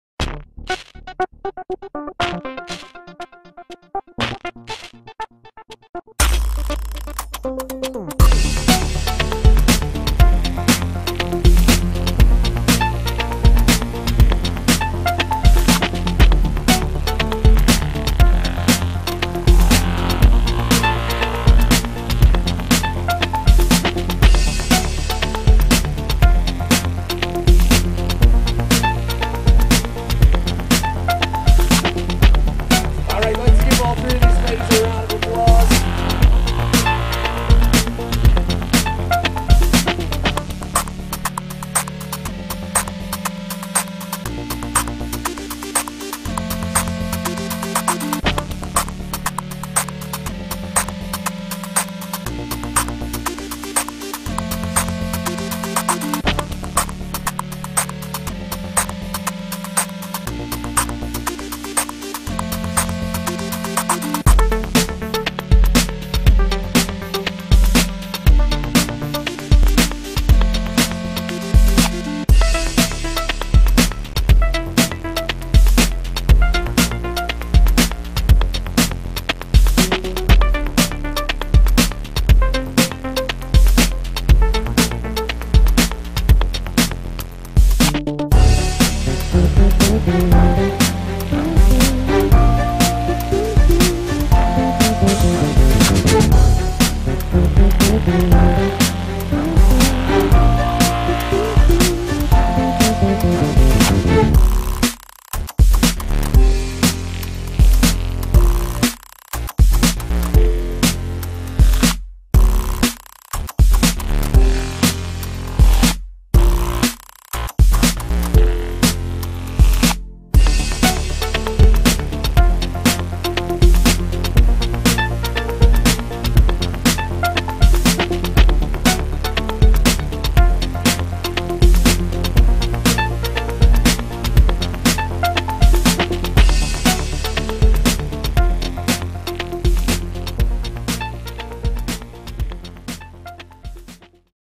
Burton Am Series
A lil recap of the Burton Amateur Series that came to Waterville.